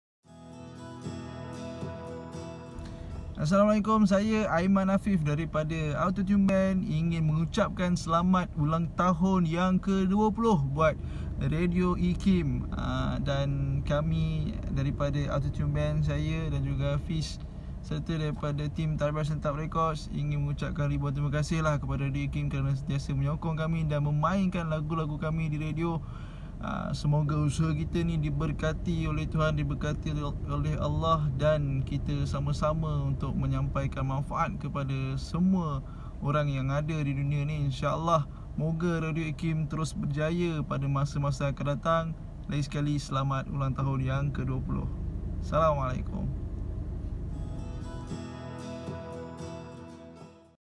Ucapan Sambutan Ulangtahun Radio IKIM ke 20 Tahun daripada Saudara Aiman mewakili Autotune Band.
20 tahun Radio IKIM bersiaran. Terima kasih kerana terus mendengarkan kami.

"Radio IKIM 20 Tahun - Inspirasi, Inforia, Islami"